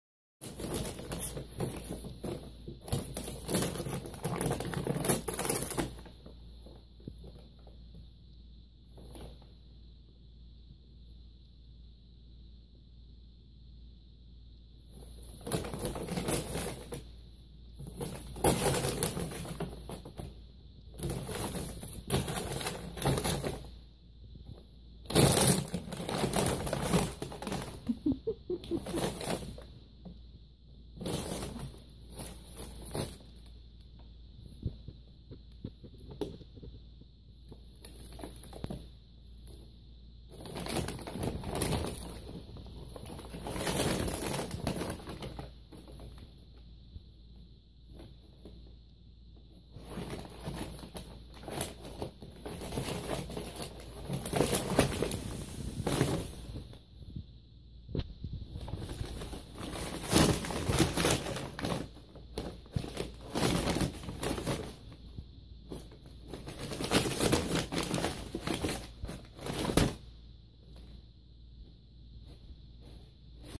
Calico In A Paper Bag
Little Cat finds some catnip in a paper bag